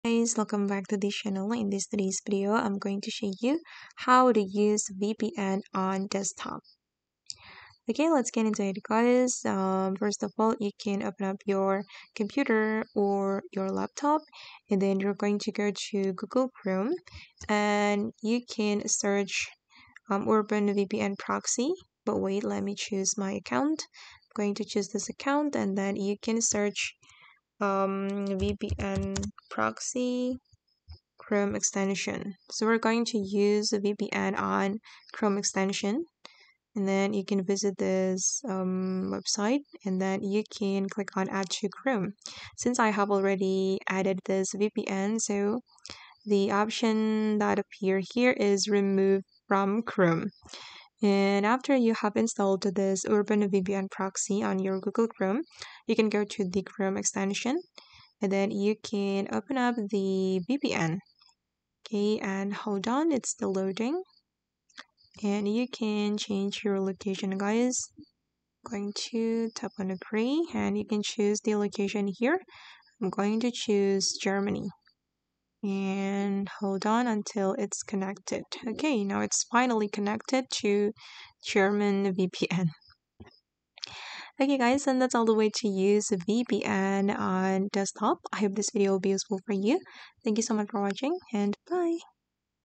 FREE VPN TRICK! 🌍 How to Use Urban VPN on Desktop Chrome Extension (Step-by-Step)
Learn how to browse safely and change your location easily using Urban VPN on your desktop! This quick tutorial shows how to install, connect, and use a VPN through the Chrome extension.
💻 Step-by-step setup for Urban VPN on Google Chrome
🌐 How to connect and change your location instantly
🧠 Tips for staying safe and browsing anonymously
🚀 Works for both PC and laptop users